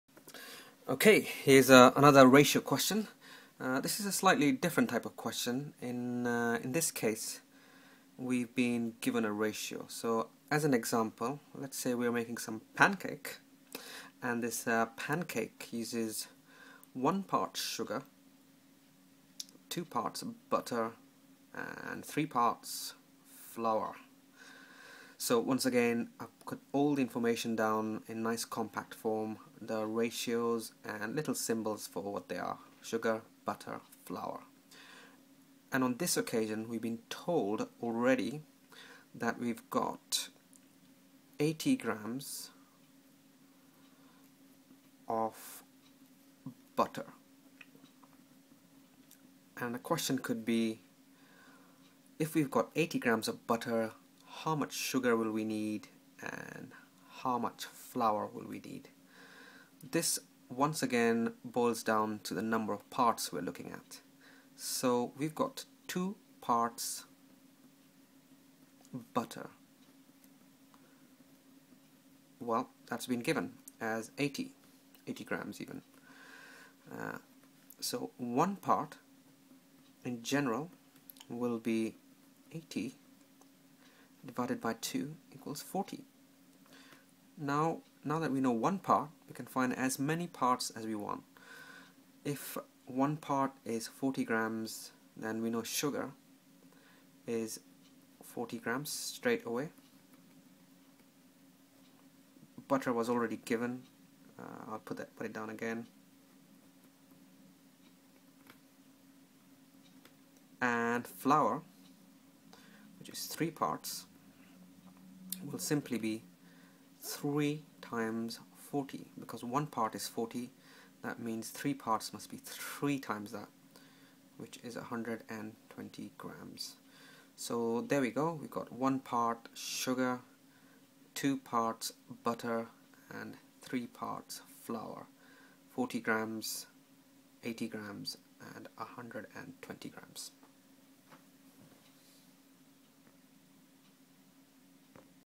Ratio and Proportion - Ratio and One Part Given, find other parts
A cake is made using sugar butter and flour in the ratio 1:2:3. 80 grams of butter  is used. How much sugar and flour is needed in grams? This video shows how to solve this problem really simply.

Dr. Atul Rana iGCSE maths tutor teaching online.